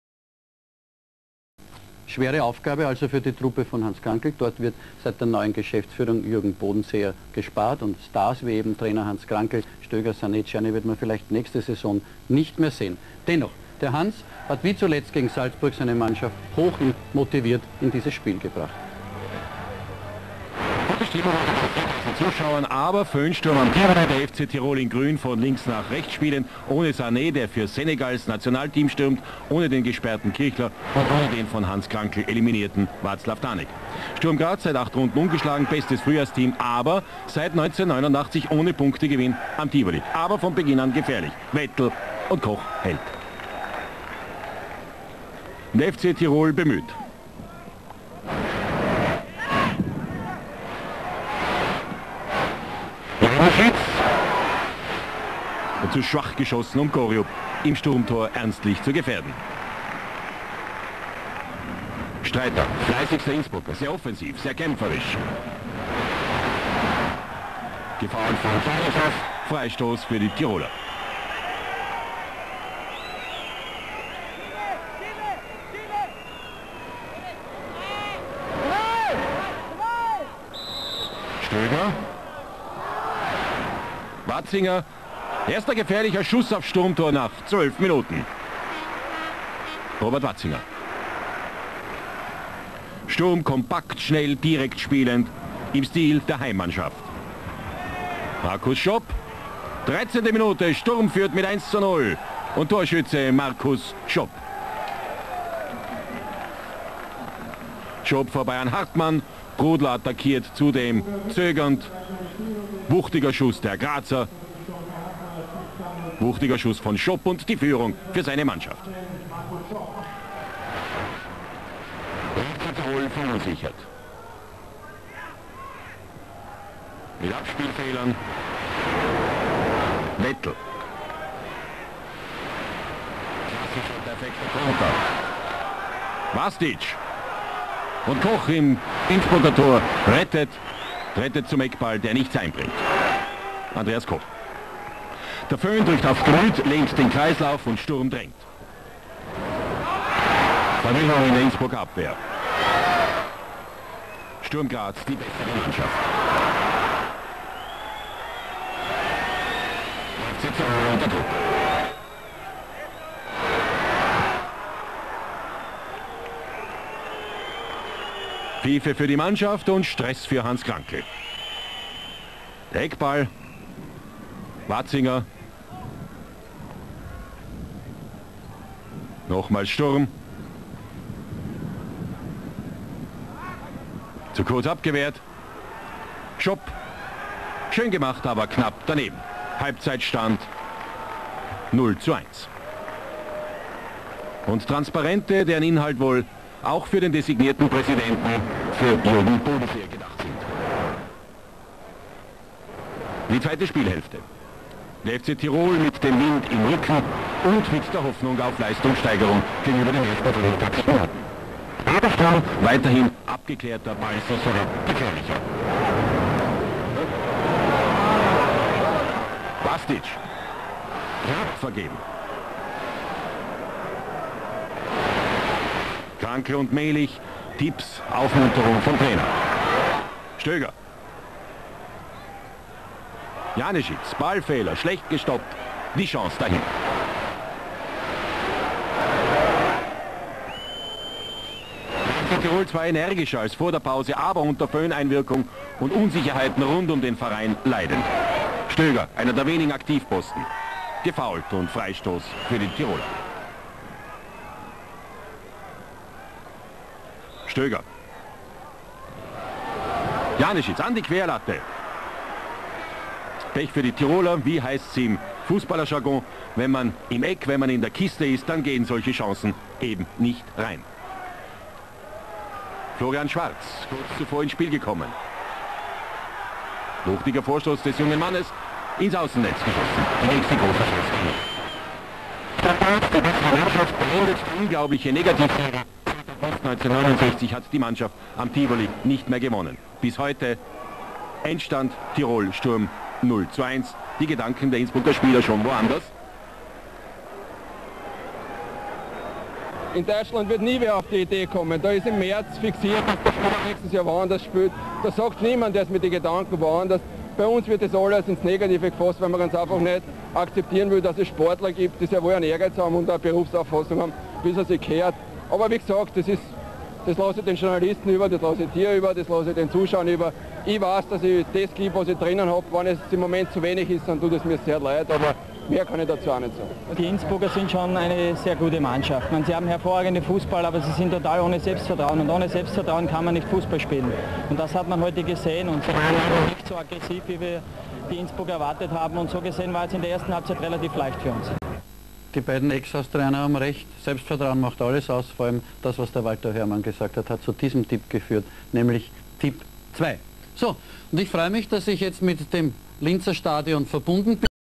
FC Tirol Innsbruck - Sturm Graz 0:1 - Saison 1994/95
FC Tirol Innsbruck Samstag, 22. April 1995
15:30 Uhr Sturm Graz
FC Tirol Innsbruck
0:1
Sturm Graz
Tore
0 : 1 Markus Schopp 14.

1 Andreas Koch
2 Jürgen Hartmann  gelb 27'
3 Michael Streiter
4 Robert Wazinger
8 Michael Baur
14 Oliver Prudlo  gelb 53'
10 Peter Stöger
12 Roman Mählich
19 Thomas Silberberger 51'
7 Harald Cerny 75'
18 Thomas Janeschitz
Reservespieler
5 Andreas Poiger 75'
15 Florian Schwarz 51'
1 Roland Goriupp
3 Andrey Chernyshov
3 Mario Posch
13 Günther Neukirchner
8 Gilbert Prilasnig
10 Walter Hörmann
12 Herbert Grassler  gelb 34'
18 Markus Schopp
7 Mario Haas
11 Ivica Vastić
15 Arnold Wetl  gelb 10' 88'
Reservespieler
17 Marchanno Schultz 88'

Trainer: Hans Krankl Trainer: Ivica Osim

Stadion Tivoli (Innsbruck / Österreich)
Zuschauerzahl 5.000
Schiedsrichter Alfred Wieser (Österreich)
Schiedsrichter-Assistent Anton Aichholzer (Österreich)
Schiedsrichter-Assistent Martin Hambaumer (Österreich)